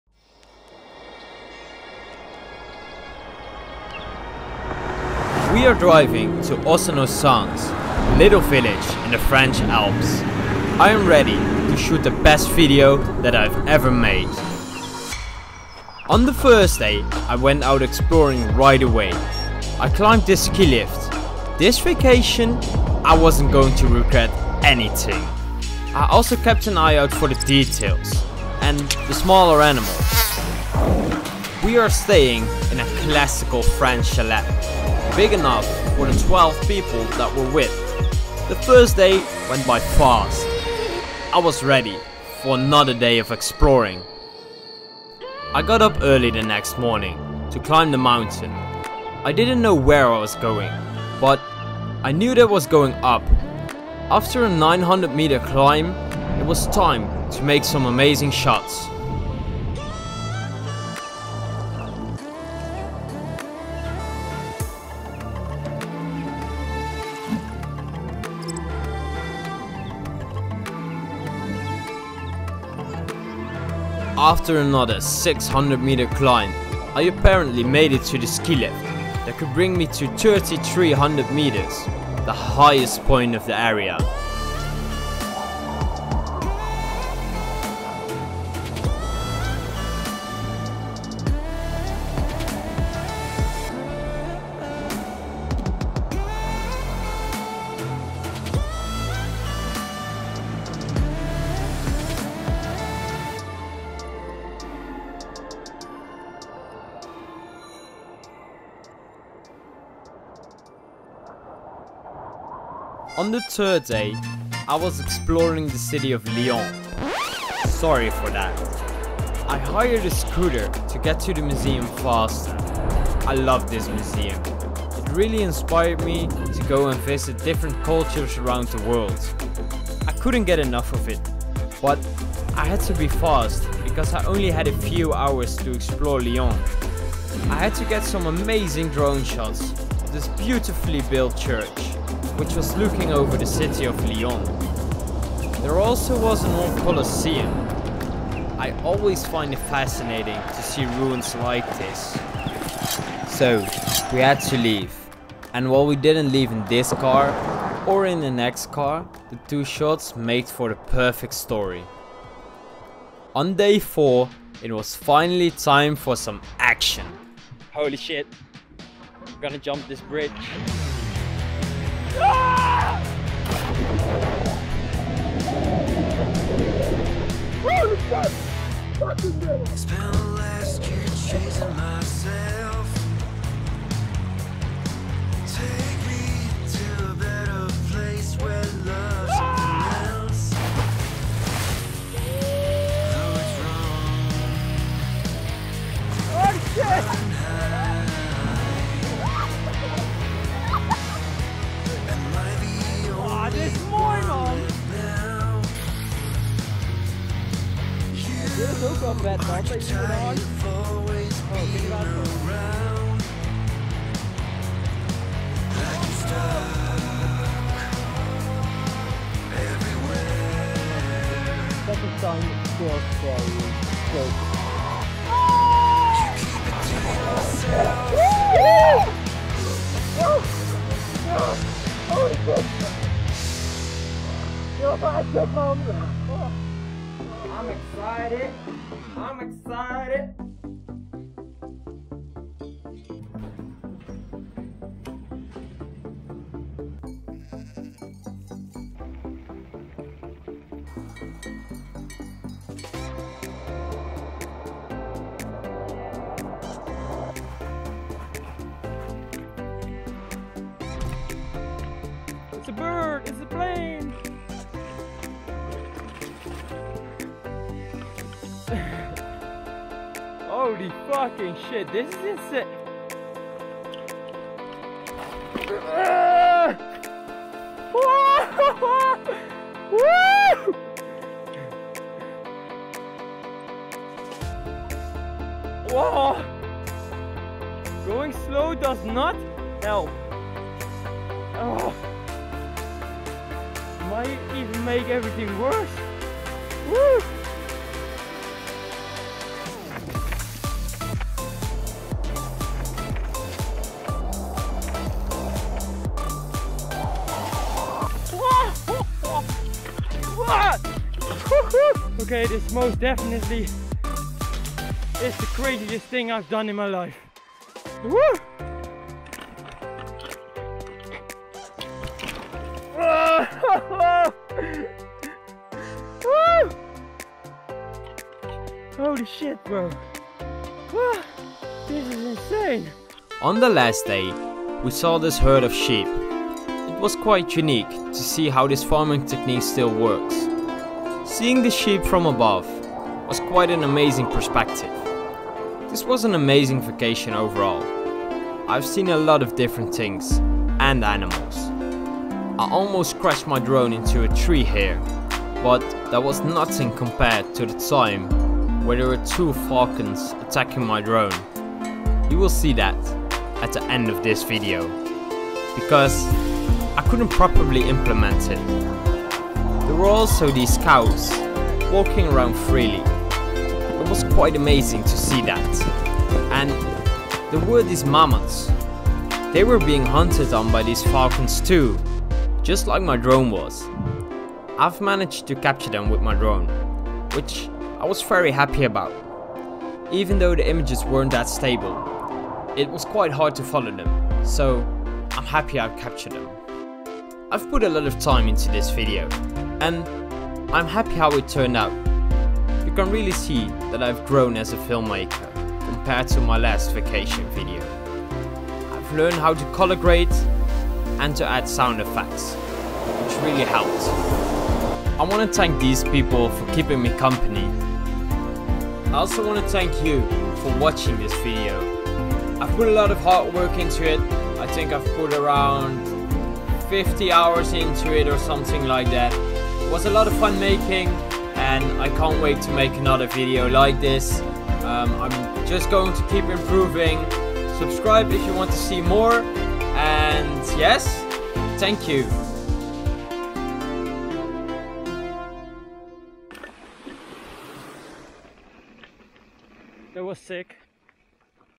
Cinematic vlog of the French alps
In this video, I am going on a vacation to the French Alps. I am in a place near alp de huez. I made some dope drone shots of the french alps.  I am going mountain biking, hiking and rafting. I saw a lot of different animals like spiders, marmots, cows and crickets. It was an incredible vacation, and I can't wait to go on a new adventure soon.

00:00 -  The French alps day 1
00:40 -  The French alps day 2
01:48 - Lyon
02:45 - Rafting & bridge jumping
03:58 - Down hill mountainbiking

I will continue to improve my videography skills up to the point that they will become memorable.

Where I get my music from: (not sponsored but I do get 1 month for free if you sign up through my link)